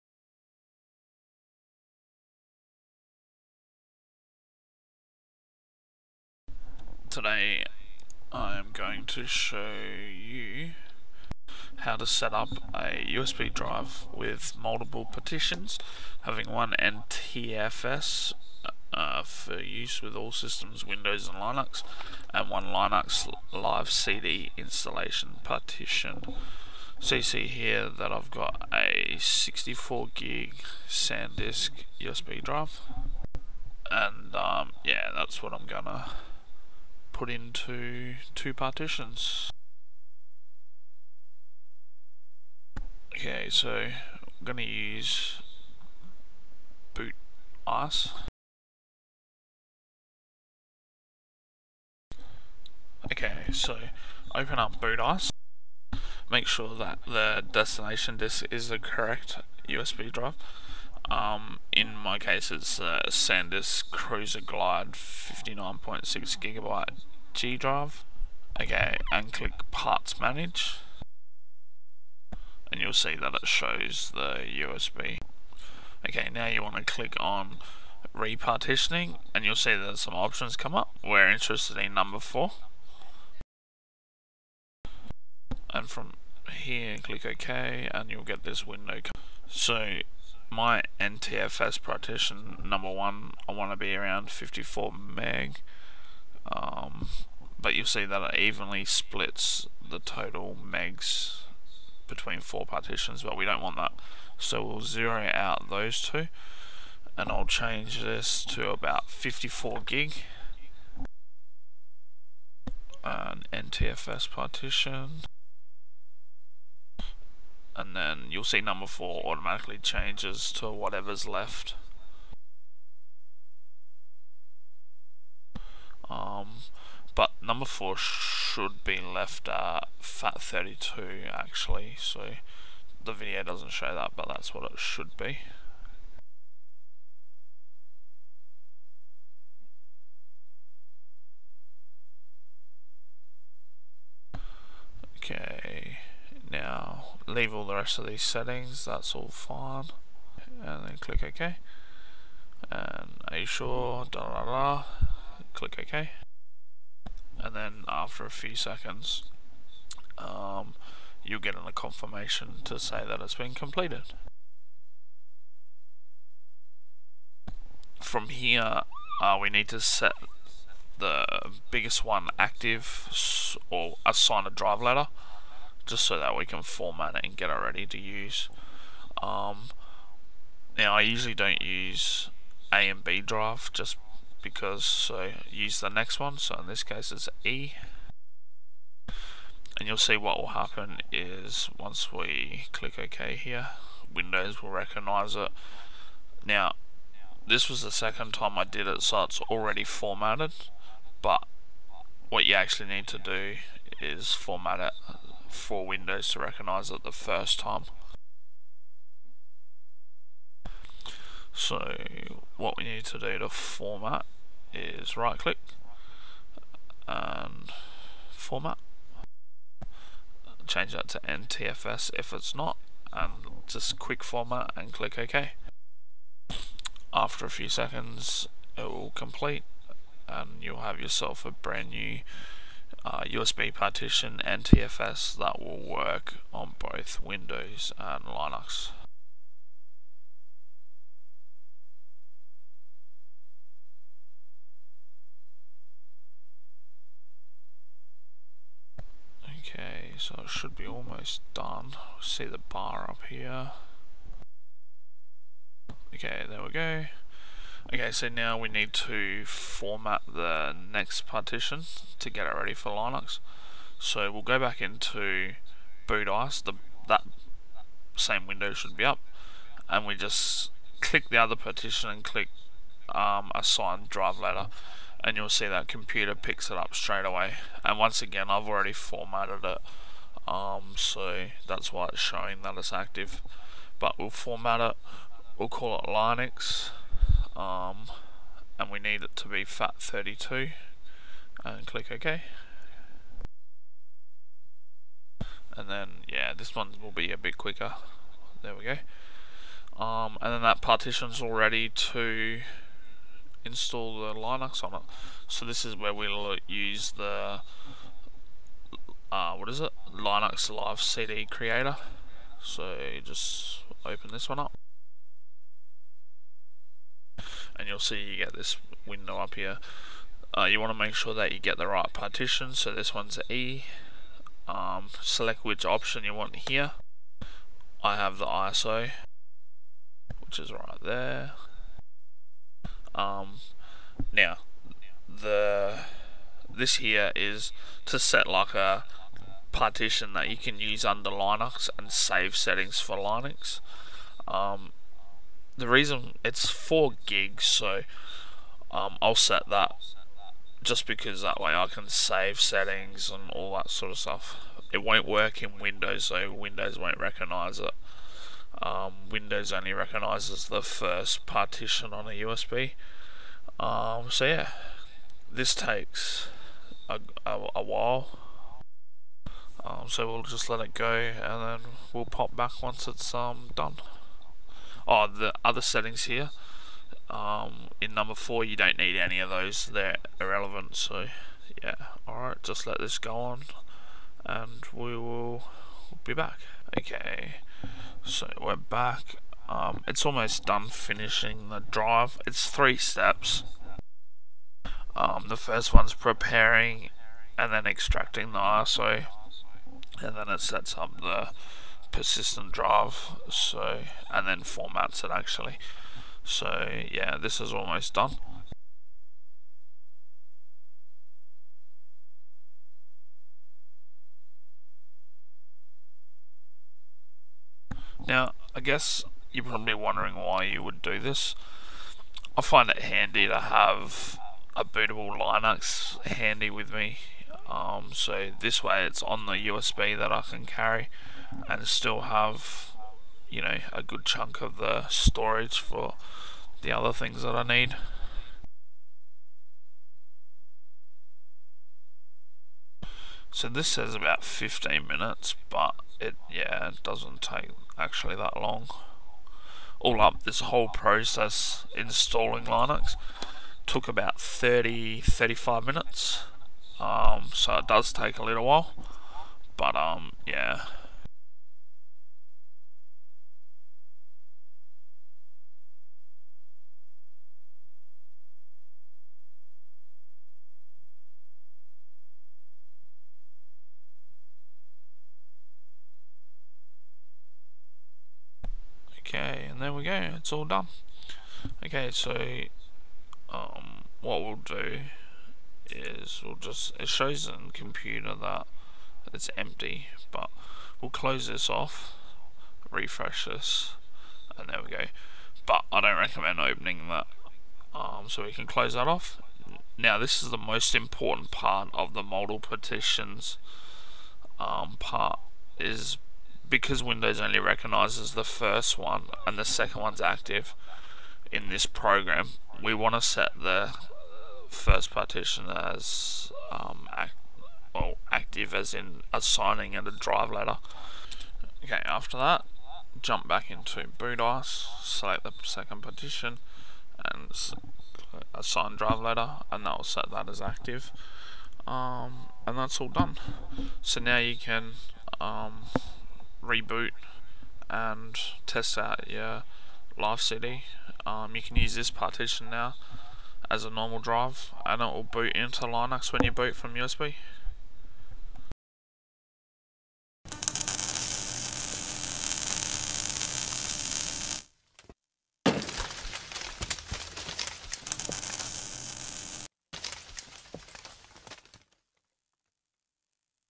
Setup USB Drive with 2 partitions. 1st NTFS for use with Win/Linux 2nd Linux Live CD from Linux iso
How do setup a USB Drive with Multiple Paritions, having 1 NTFS (For use with all systems, Windows & Linux) and 1 for a linux Live CD / installation
BACKUP EVERYTHING ON THE USB FIRST! YOU WILL LOSE IT!!!

Happy Times. Like the video , and have a nice day :)

Other info.
I was doing all this on Windows 8 Pro with WMC 64bit
Versions on the programs i was using
BootICE 1.06
LinuxLive USB Creator 2.8.23